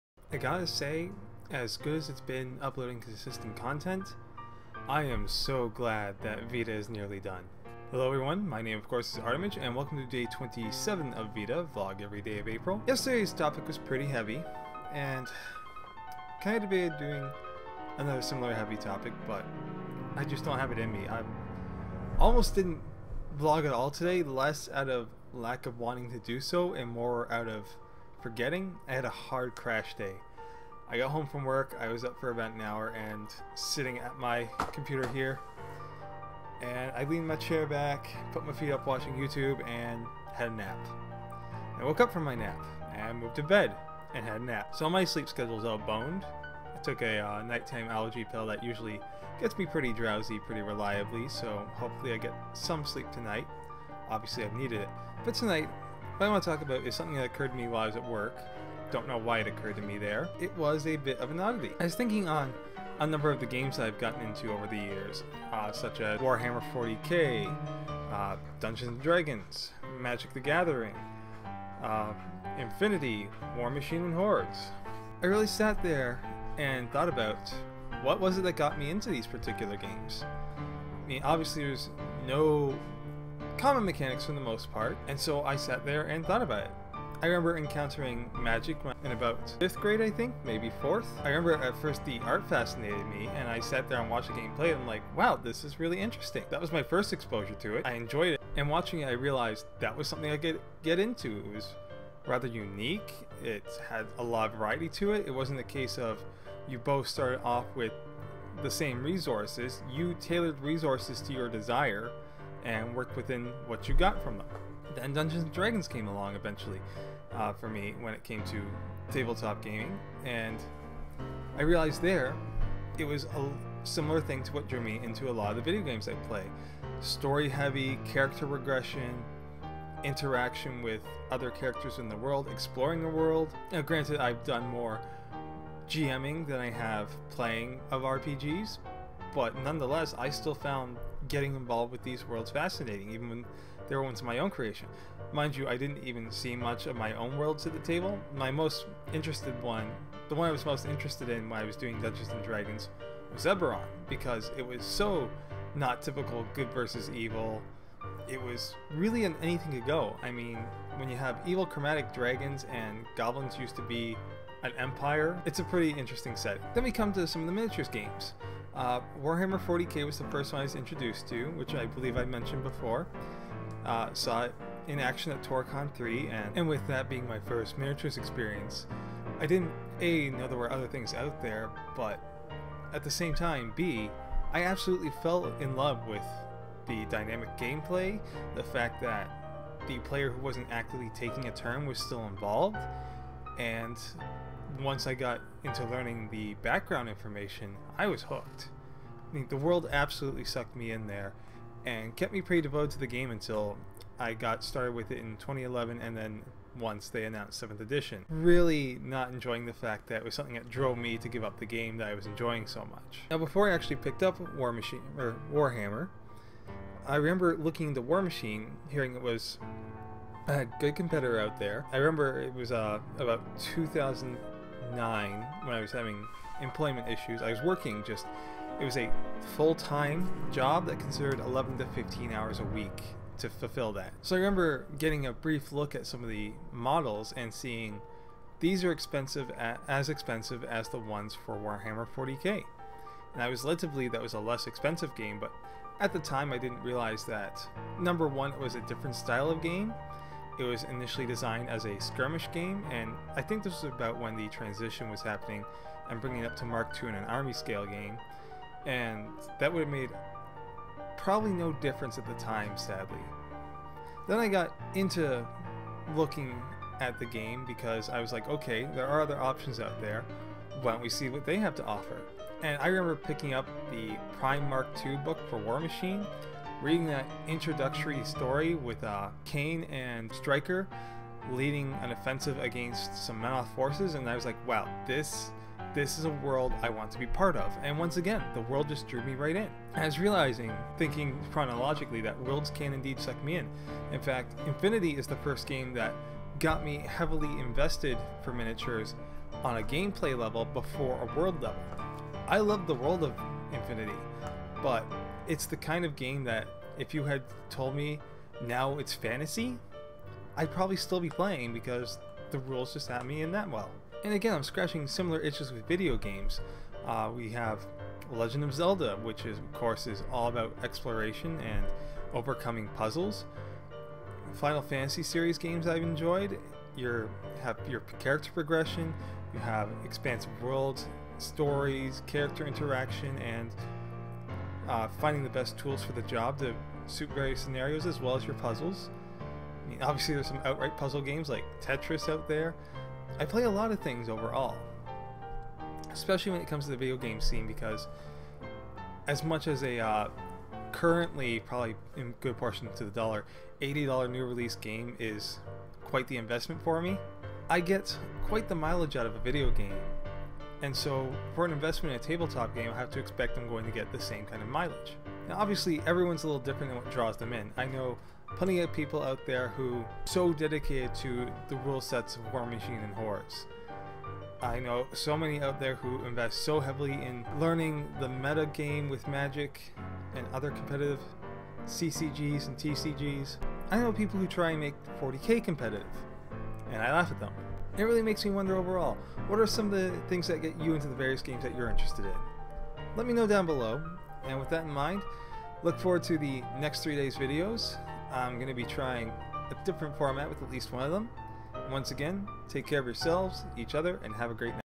VEDA Day 27 - April 27, 2017 - The Personal Allure of Games
Artimage talks today about the ways various games have drawn him in, what their specific allure is.